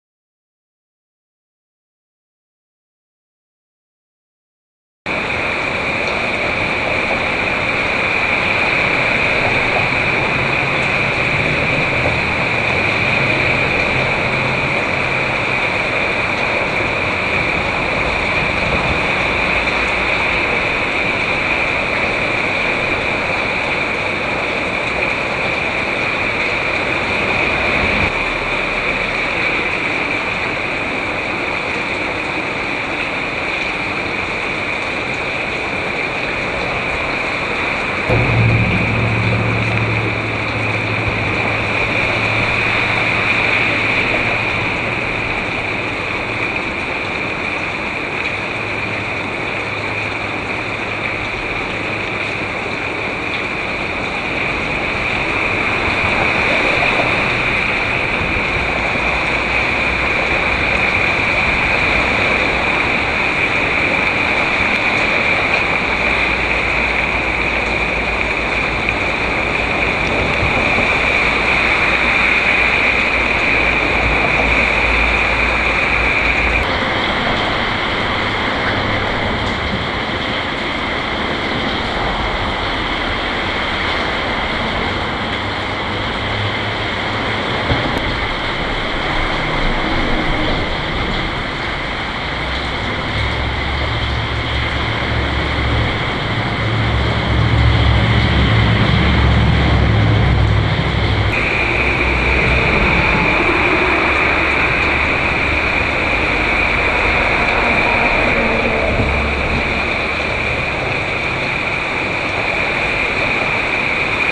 Stock Footage; Drain pipes or culverts #1
High quality 16:9 HD video of dirty water gushing from large drain pipes or culverts into Sarasota bay in Florida.  Rain water run-off, water pollution, watershed, environmental, water quality.  Native 1080i high definition stock footage.  SSM Cat#20071024a.

Subsurface Media
Producer: Brian Dombrowski